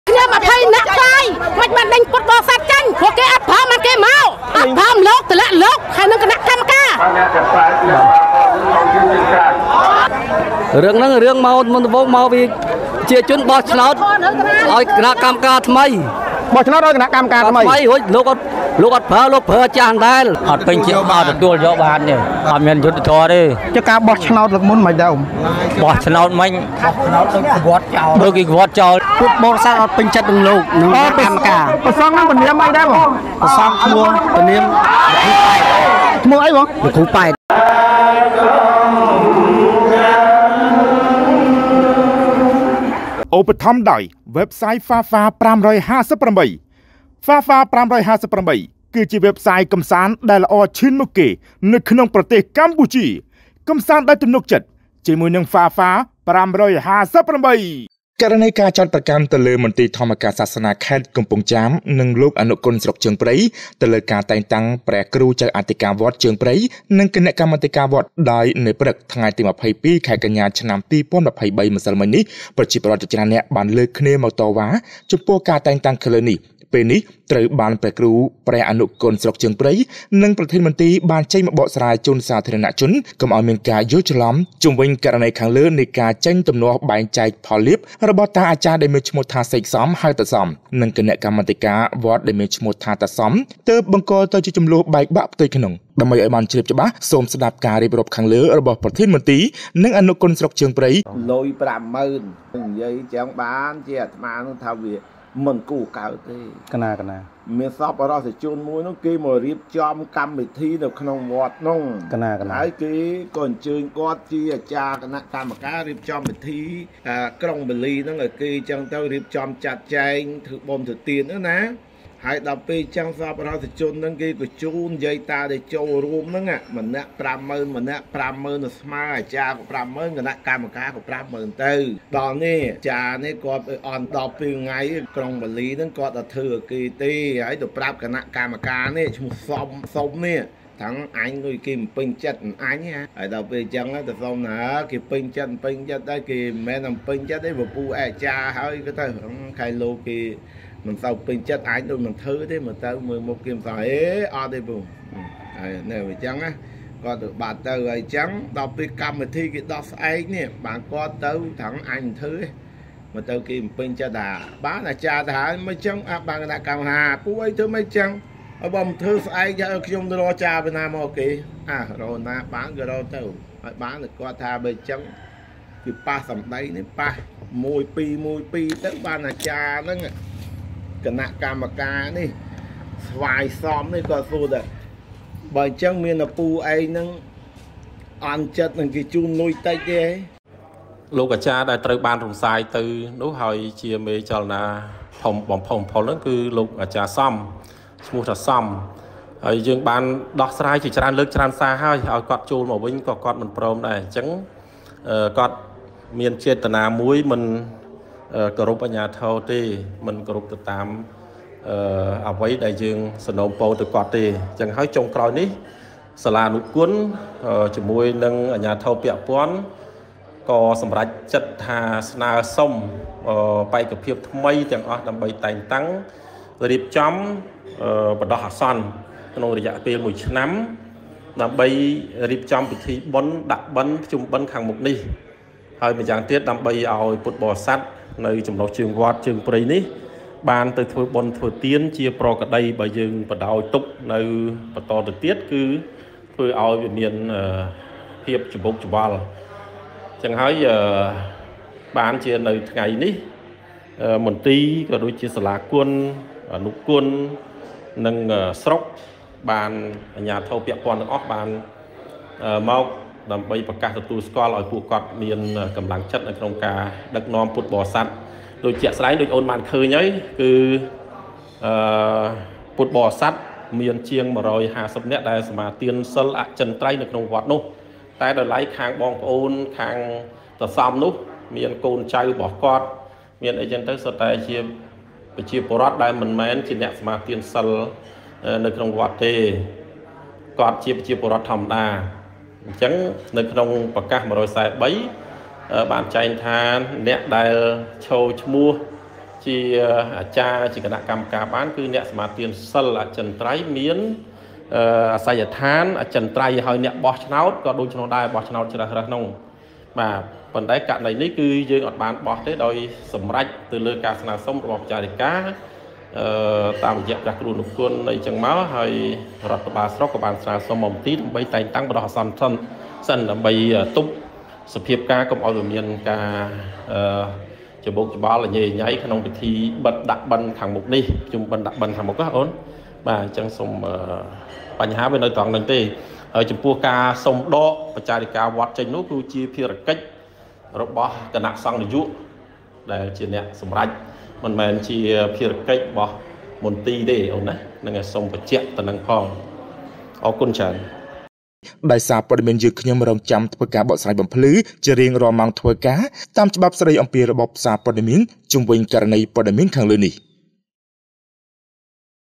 ករណីការចោទប្រកាន់ទៅលើ មន្ទីធម្មការសាសនាខេត្ត កំពង់ចាម និងអនុគុណស្រុកបាធាយ ទៅលើការតែងតាំងព្រះការអធិកា
ករណីការចោទប្រកាន់ទៅលើ មន្ទីធម្មការសាសនាខេត្ត កំពង់ចាម និងអនុគុណស្រុកបាធាយ ទៅលើការតែងតាំងព្រះការអធិការវត្តជើងព្រៃនិងគណៈកម្មាធិការវត្ត ថ្ងៃទី 22 ខែកញ្ញាកាលពីព្រឹកម្សិលមិញ ប្រជាពលរដ្ឋជាច្រើនលើកគ្នាមកតវ៉ាចំពោះការតែងតាំងខាងលើននេះ ពេលនេះត្រូវបានព្រះគ្រូអនុគណស្រុកជើងព្រៃ និង ប្រធានមន្ទី បានបកស្រាយជូនសាធារណៈជនកុំឲ្យមានការយល់ច្រឡំជុំវិញករណីខាងលើការចេញពីទំនាស់បែងចែកផលលាភ